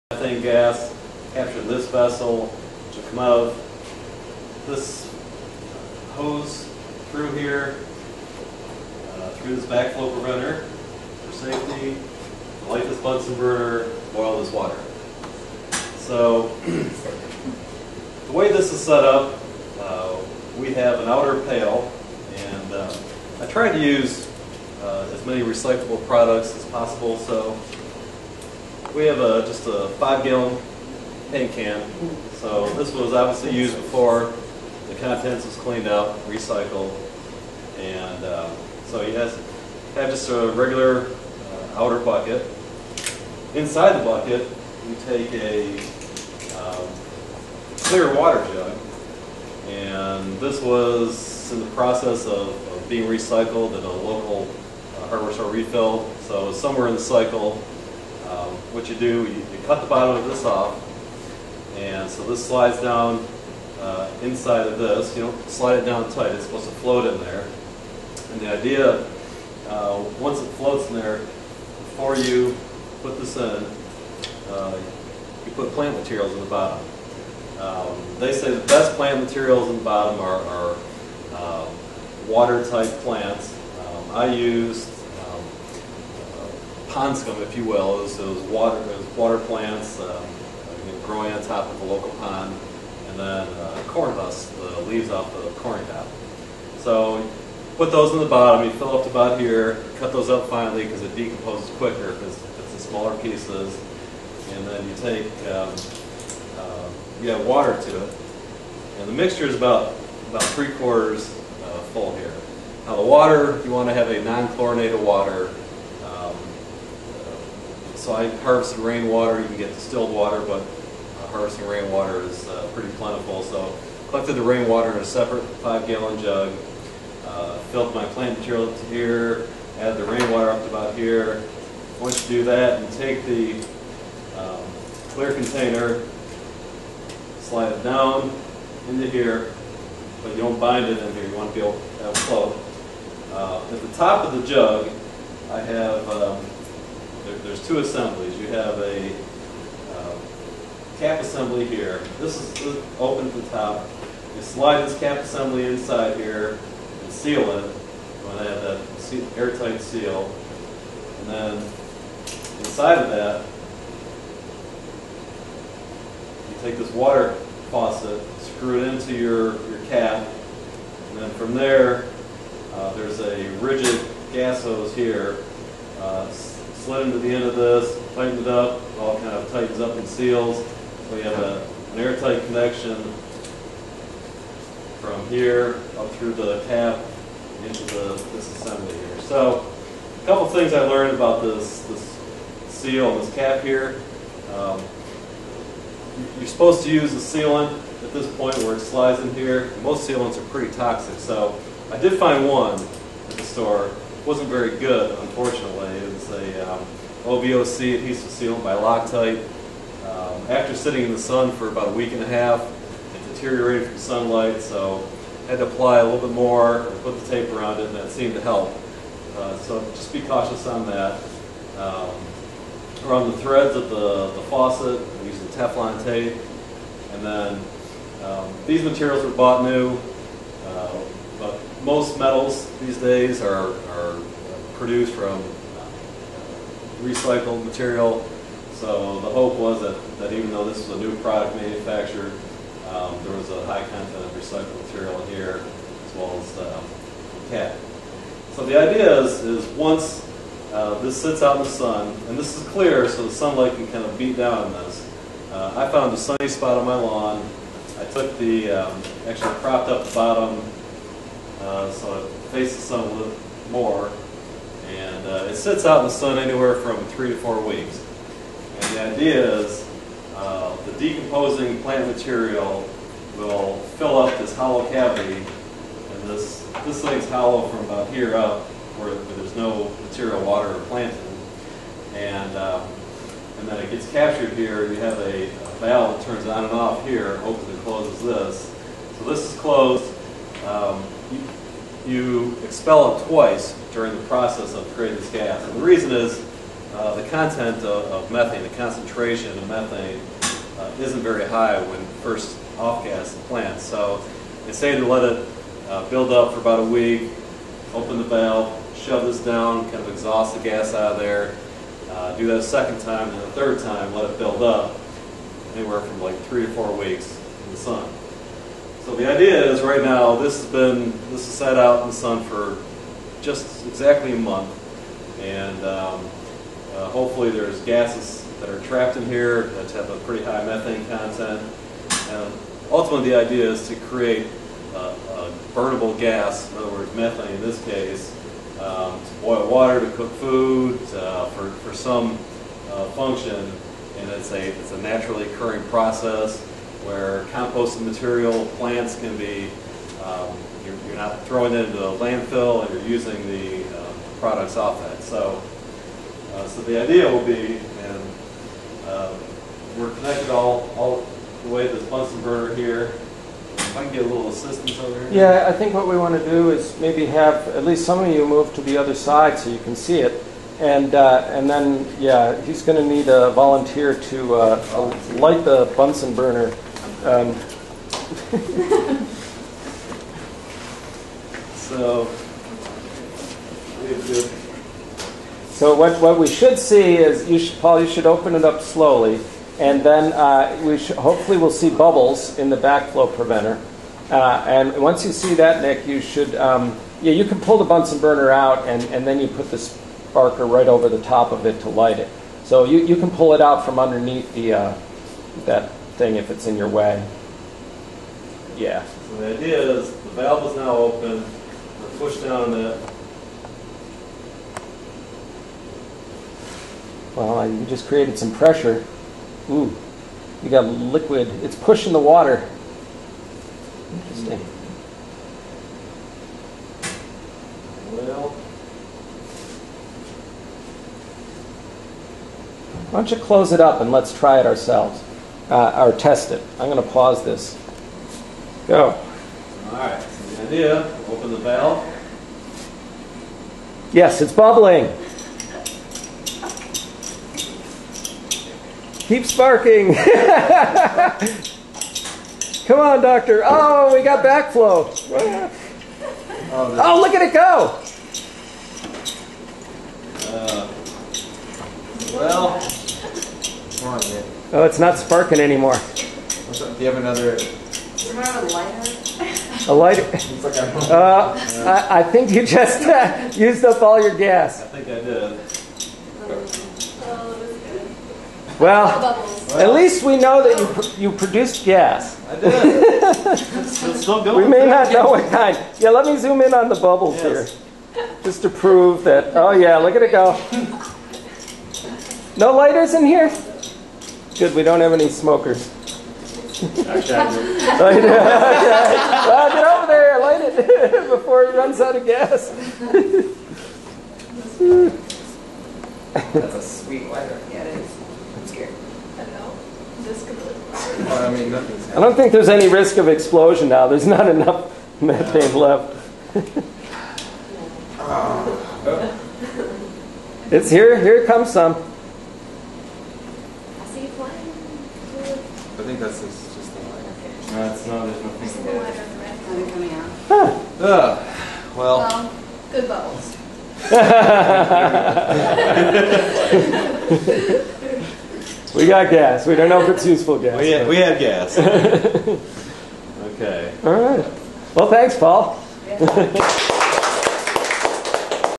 Biogas digestor
Demonstration of a system that converts plant debris to methane. Built for the course EES390 Sustainability Science taught by Professor John Ayers at Vanderbilt University.